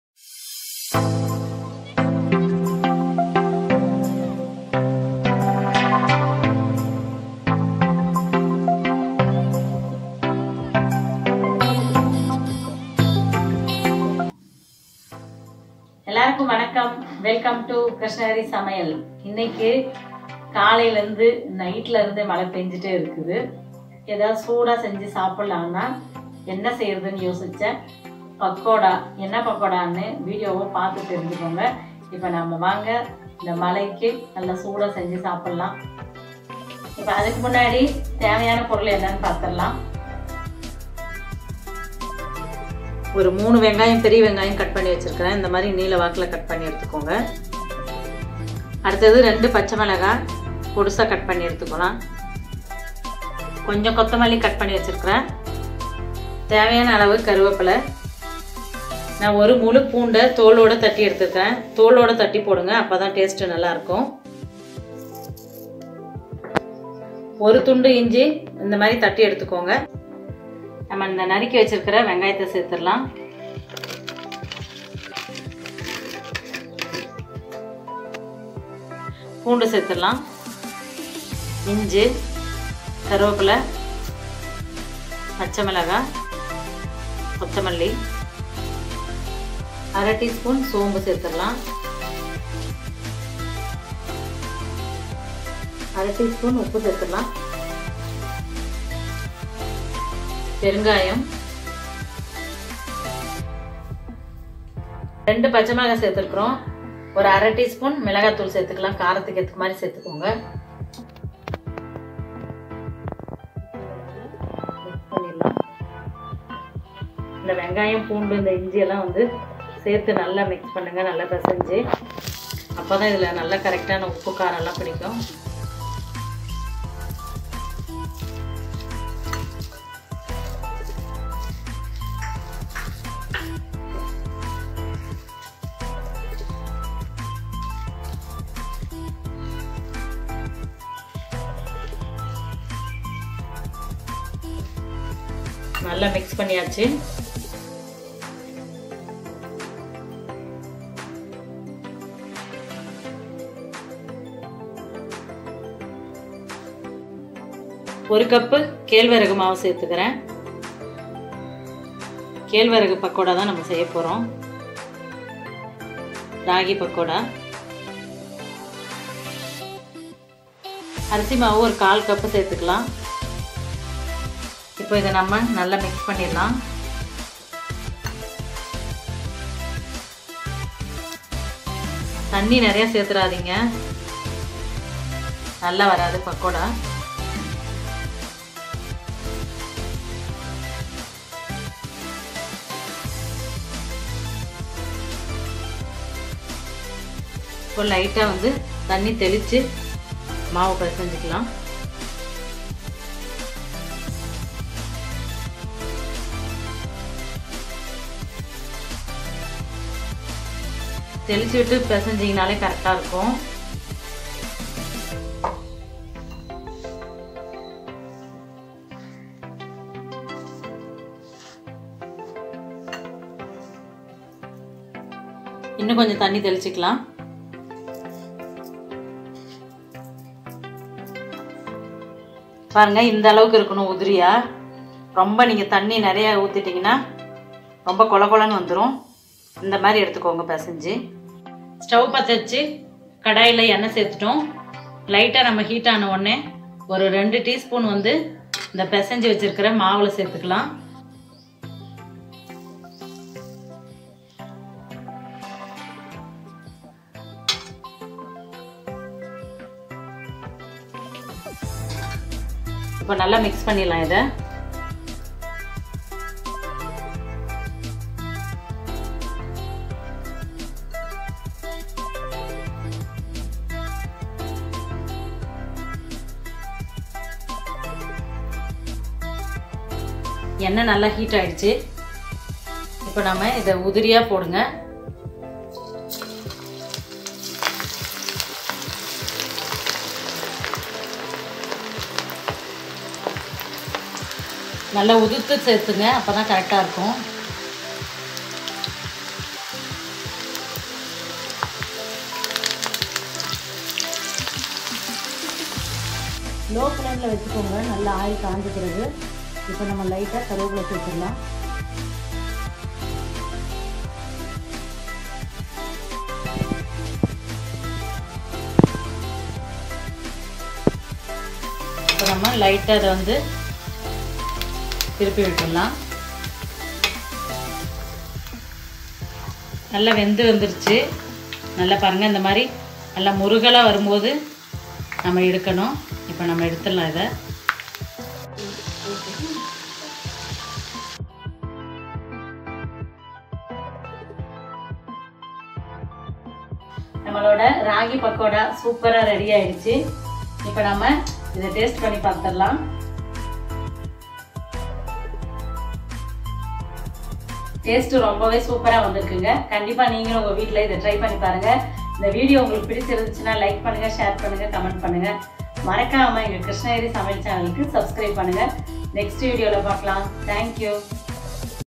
Ragi pakkoda recipe in Tamil Krishnagiri Samayal
In this video we all gonna see how to make ragi pakkoda recipe in Tamil